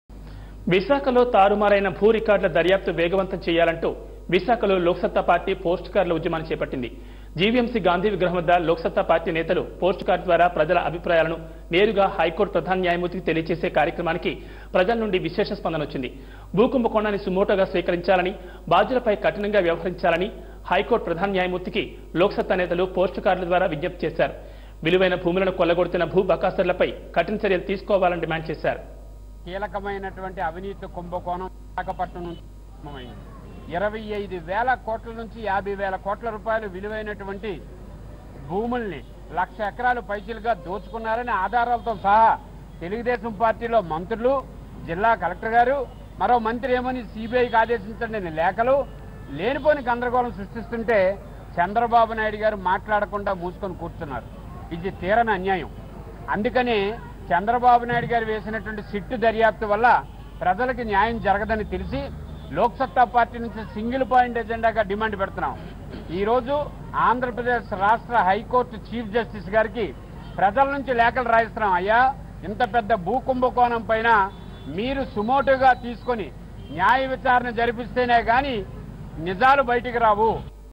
భూకుంభ‌కోణాల‌పై పోస్టు కార్డు ఉద్య‌మం
ఆంధ్ర‌ప్ర‌దేశ్ రాష్ట్రం విశాఖ‌లో బ‌య‌ట‌ప‌డుతున్న భూకుంభ‌కోణాల‌పై లోక్‌స‌త్తా పార్టీ పోస్టుకార్డు ఉద్య‌మాన్ని ప్రారంభించింది.